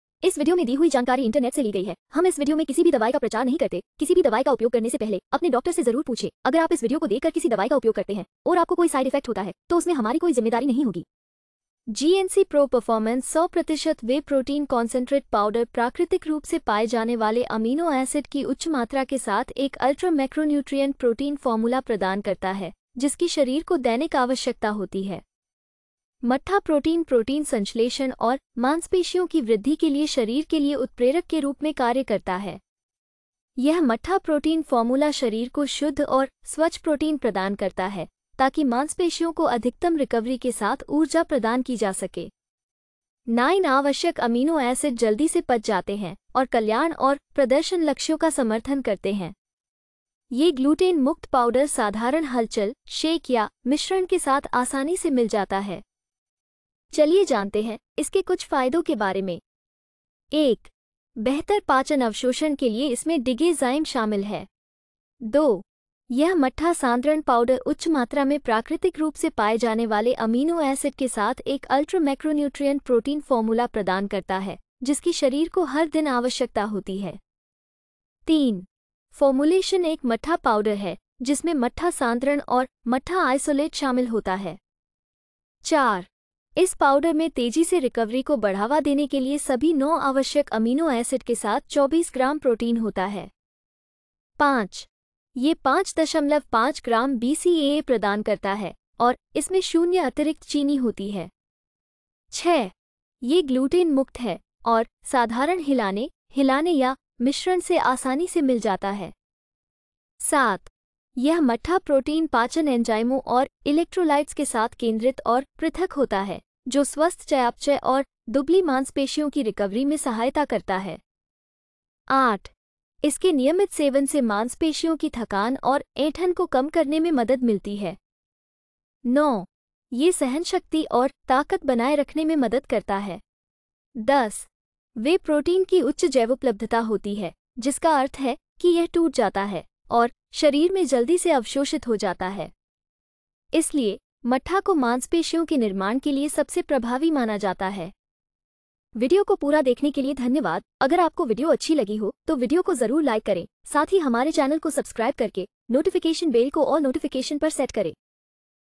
GNC Pro Performance 100% Whey Protein - With Digestive Enzymes and Electrolytes - For Metabolism and
GNC Pro Performance 100% Whey Protein -  With Digestive Enzymes and Electrolytes - For Metabolism and Lean Muscles Recovery - Flavour Powder Chocolate Fudge Uses in Hindi | Benefits | Side Effects | Dose